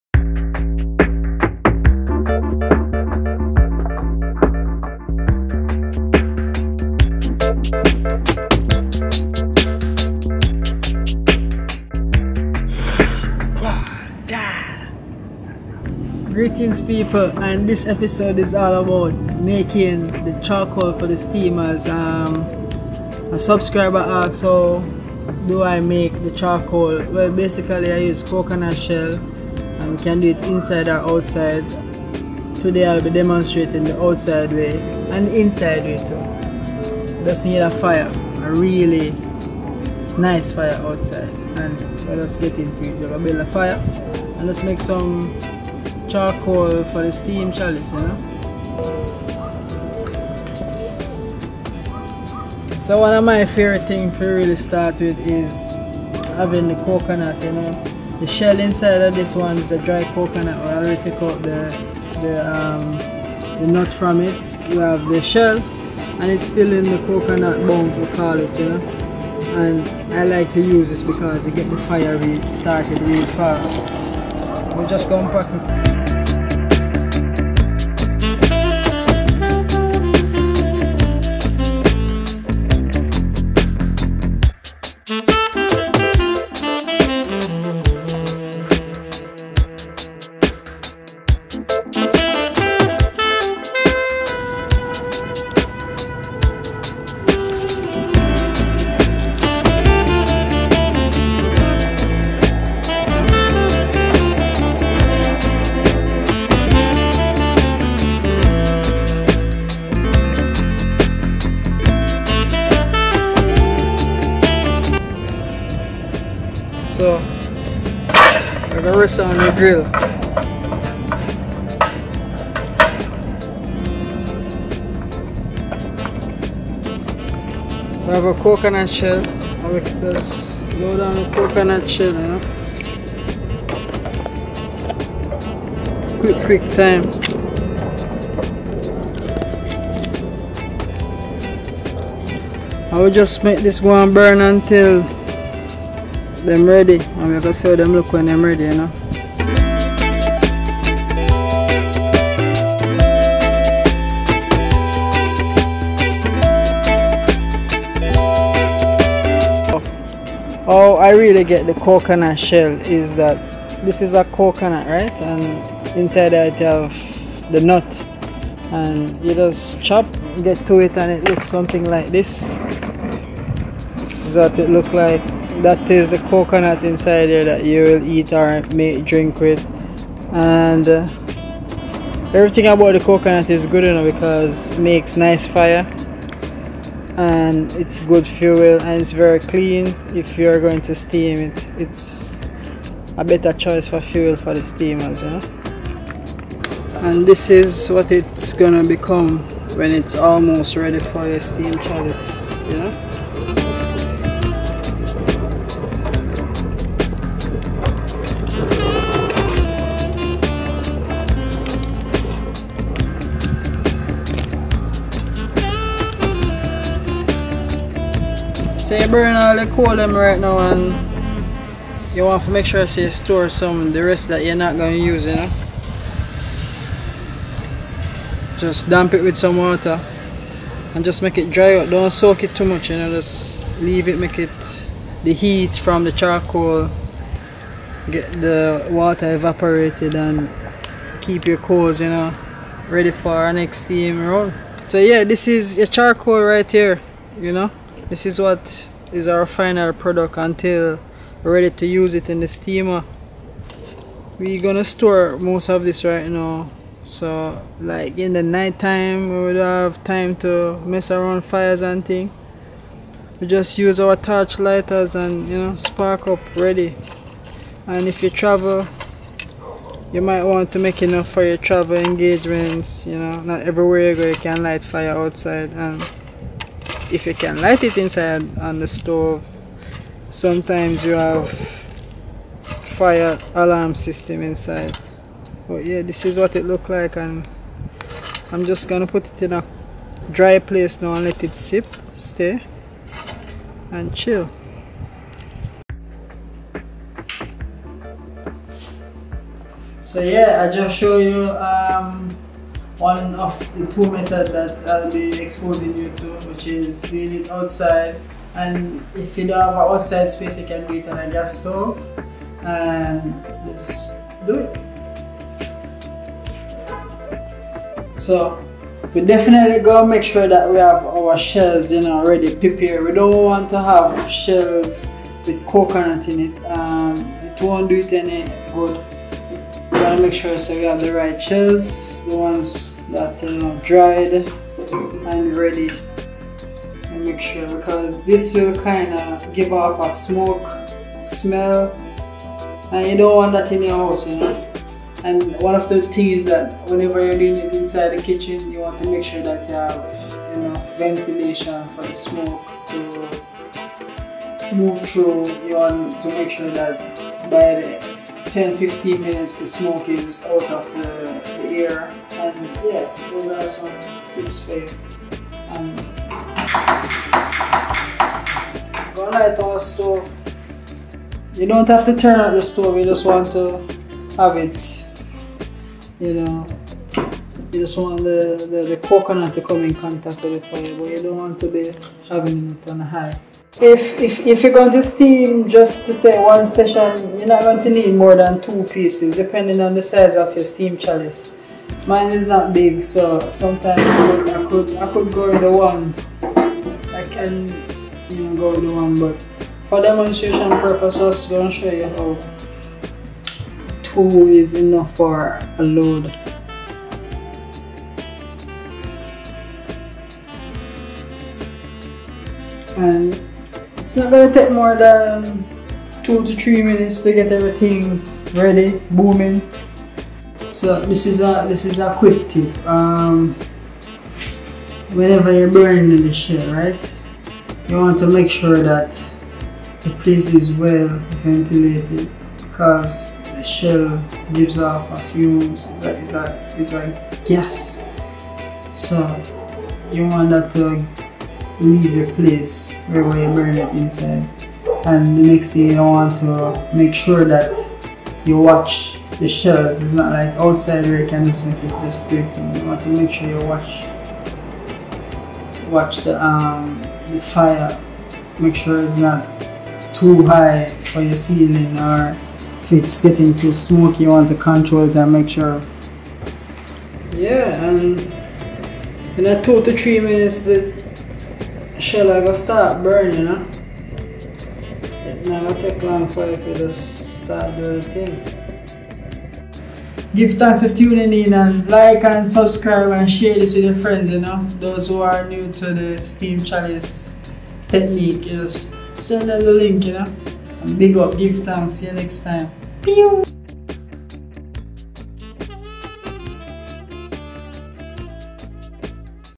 How to make COCONUT CHARCOAL for your Marijuana STEAM CHALICE
Greetings, here I show you how to make the coconut charcoal for your marijuana steam chalice. Both indoor and outdoor methods.

Using coconut shell is one of the best ways to burn your steamers.